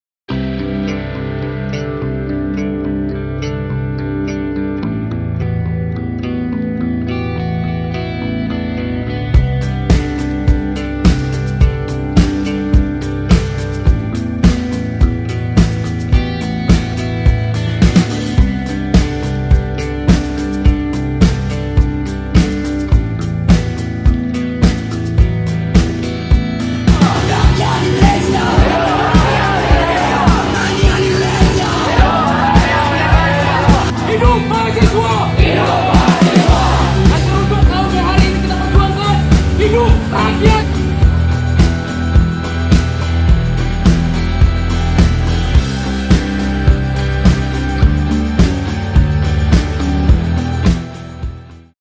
Hari Tani Nasional, Ratusan Mahasiswa Demo di Depan Kantor Gubernur Sumbar
Ratusan mahasiswa yang berasal dari berbagai universitas di Kota Padang, Sumatra Barat (Sumbar) menggelar aksi demonstrasi di depan Kantor Gubernur Sumbar, Kamis (24/9/2020). Mereka menyuarakan kesejahteraan petani pada momentum Hari Tani Nasional yang diperingati hari ini.

Selengkapnya